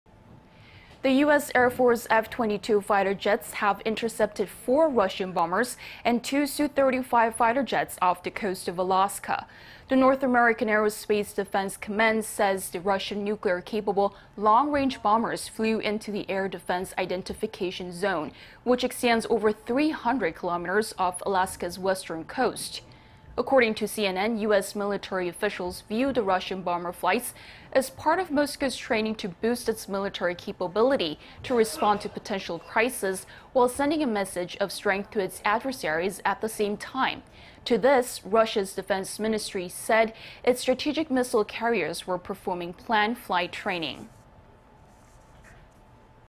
U.S. F-22 fighter jets intercept 4 Russian bombers, 2 fighter jets off coast of Alaska
美공군, 알래스카 해안 근접비행 러시아 전폭기 등 6대 차단

The U.S. Air Force F-22 fighter jets have intercepted four Russian bombers and two Su-35 fighter jets off the coast of Alaska.
The North American Aerospace Defense Command says the Russian nuclear capable long-range bombers flew into the Air Defense Identification Zone,... which extends over three-hundred kilometers off Alaska's western coast.
According to CNN,... U.S. military officials view the Russian bomber flights as part of Moscow's training to boost its military's capability to respond to a potential crisis while sending a message of strength to its adversaries at the same time.
To this,... Russia's defense ministry said its strategic missile carriers were performing planned flight training.